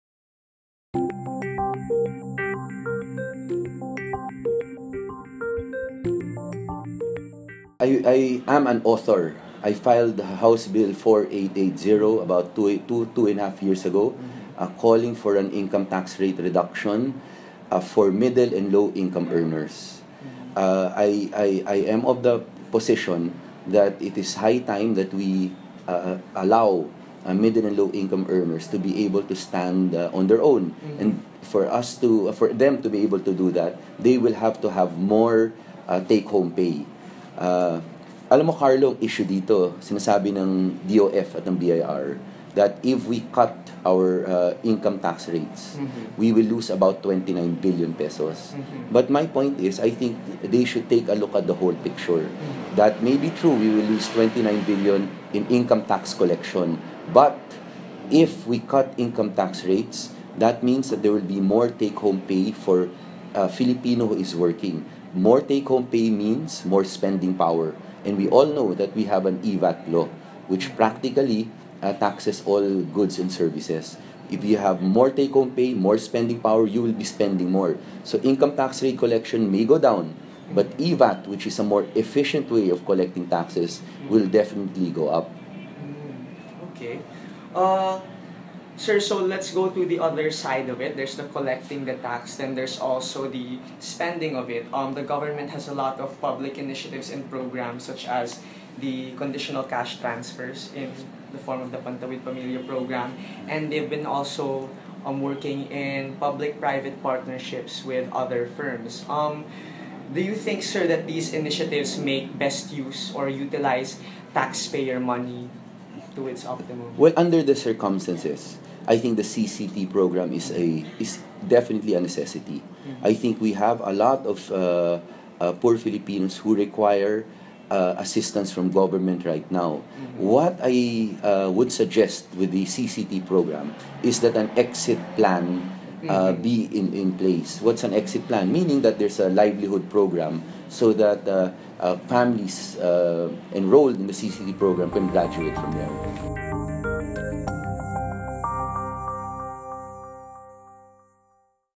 Roman Romulo on tax reform & CCT
Watch senatorial candidate Roman Romulo talk about his advocacies, his perspectives on issues of the Philippine Economy, and his overall vision for the country.

TOPVOTE 2016 is the official 2016 Philippine National Elections campaign initiative of the UP School of Economics Student Council.